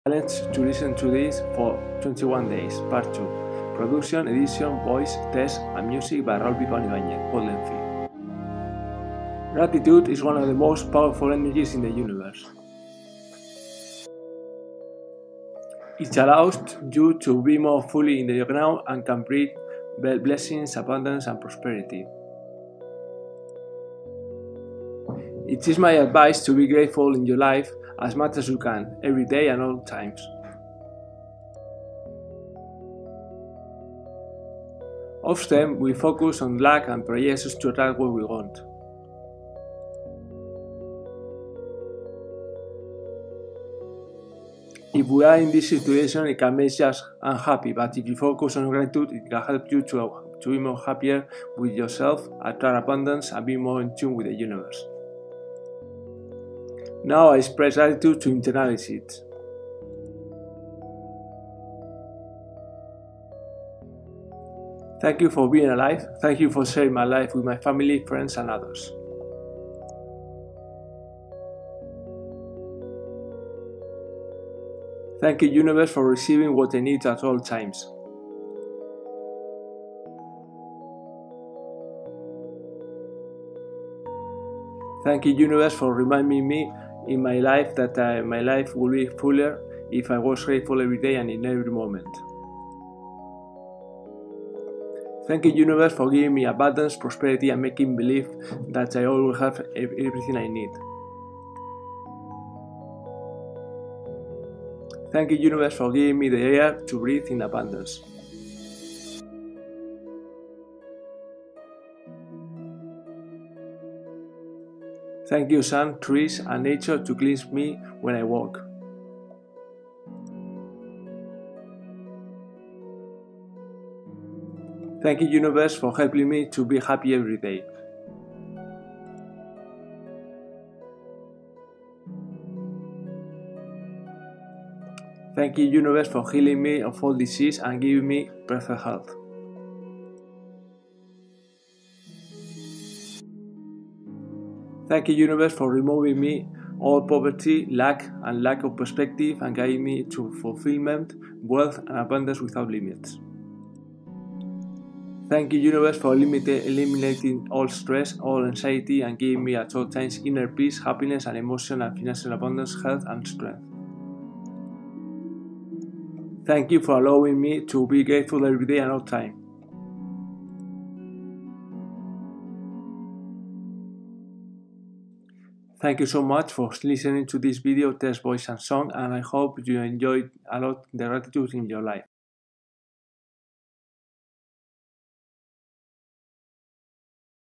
POWERFUL AFFIRMATIONS OF GRATITUDE.CHALLENGE TO LISTEN 21 DAYS #41.- raúl pipaon ibañez - goldenphi
Gratitude is one of the most powerful energies in the universe. It allows you to be more fully in the now and can bring blessings, abundance and prosperity. It is my advise to be grateful in your life as much as you can, every day and at all times. Often we focus on lack and projections to attract what we want. If we are in this situation it can make us unhappy, but if you focus on gratitude, it can help you to be happier with yourself, attract abundance and be more in tune with the universe. Now I express gratitude to internalize it:
Thank you for being alive, thank you for sharing my life with my family, friends and others. Thank you universe for receiving what I need at all times. 9 Thank you universe for reminding me in my life that my life would be fuller if I was grateful every day and in every moment. Thank you universe for giving me abundance, prosperity and making me believe that I always have everything I need. Thank you universe for giving me the air to breathe in abundance. Thank you sun, trees and nature to cleanse me when I walk. Thank you universe for helping me to be happy every day. Thank you universe for healing me of all disease and giving me perfect health. 15 Thank you universe for removing all poverty, lack and lack of perspective and guiding me to fulfillment, wealth and abundance without limits. 16 Thank you universe for eliminating all stress, all anxiety and giving me at all times inner peace, happiness and emotional and financial abundance, health and strength. Thank you for allowing me to be grateful every day and all the time. Thank you so much for listening to this video, text, voice and song and I hope you enjoy a lot the gratitude in your life. THE END.